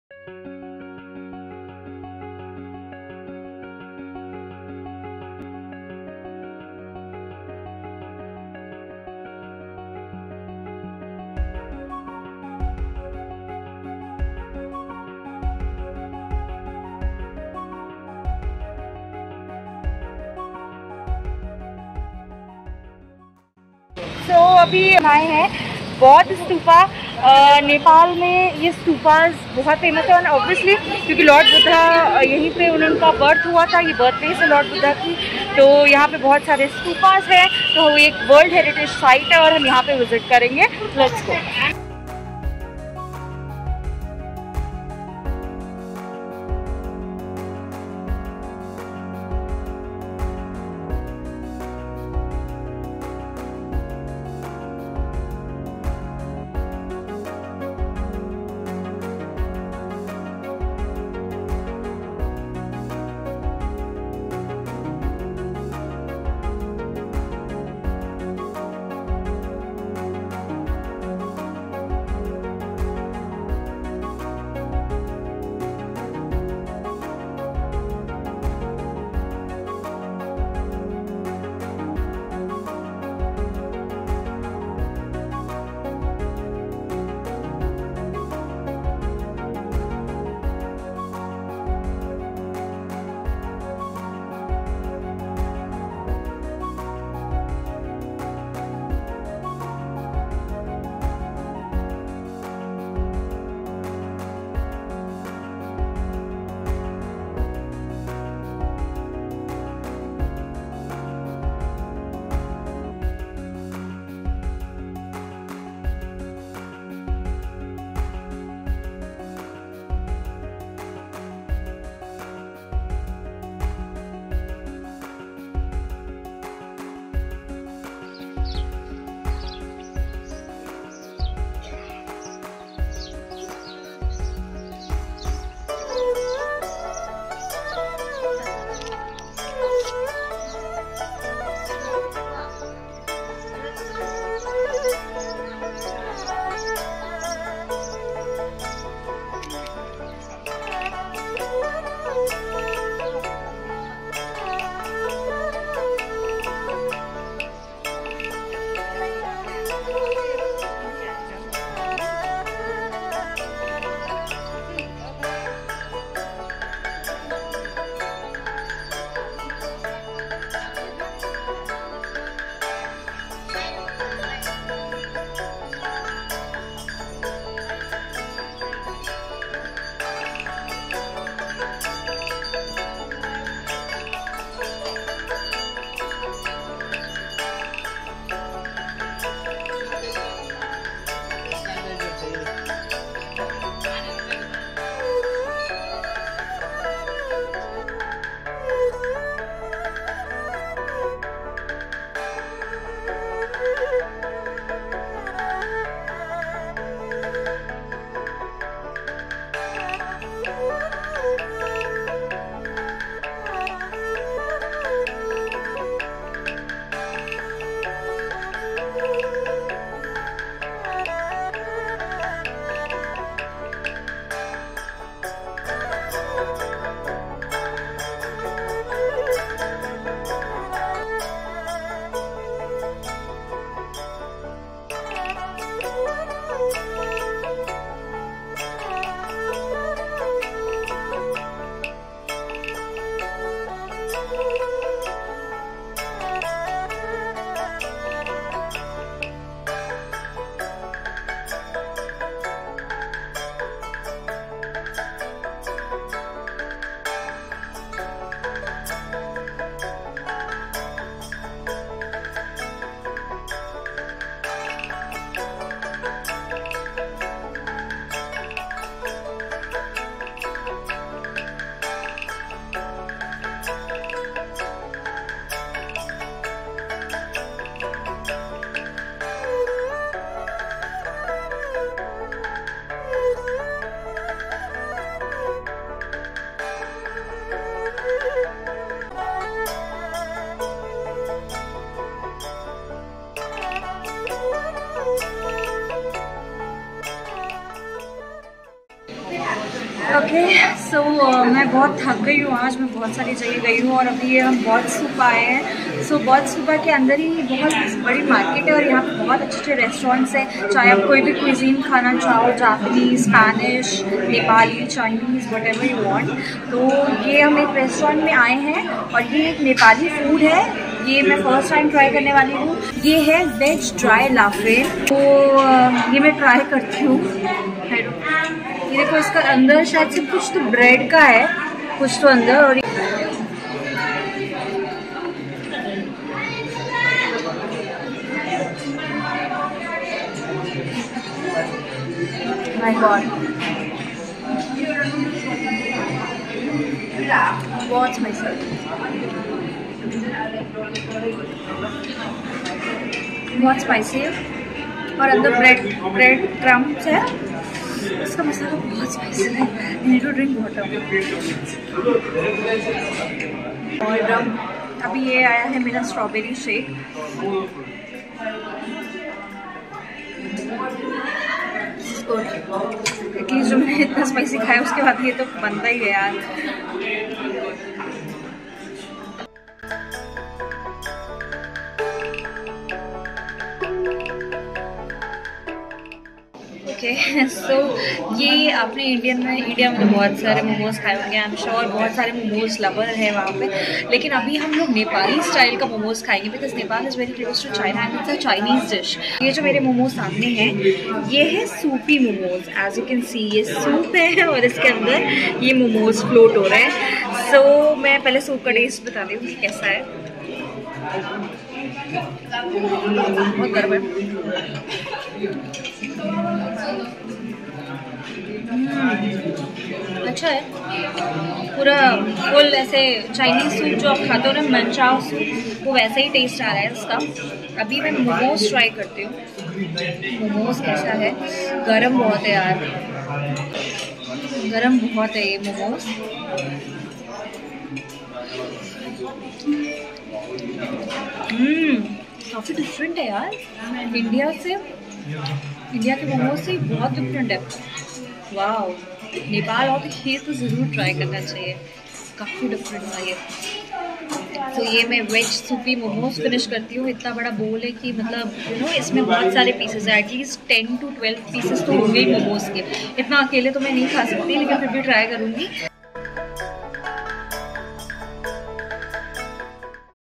Boudhanath | Boudh Stupa, Nepal | Trying The BEST TIBETAN-NEPALI FOOD in Nepal | Laphing - Ep 4
The Boudha Stupa is a UNESCO World Heritage Site in Kathmandu, Nepal. It dominates the the skyline of the Kathmandu City and is also one of the largest stupas in the world. The influx of large populations of refugees from Tibet has seen the construction of over 50 gompas (Tibetan convent) around Boudha.  It is one of the most popular tourist attractions in Kathmandu city.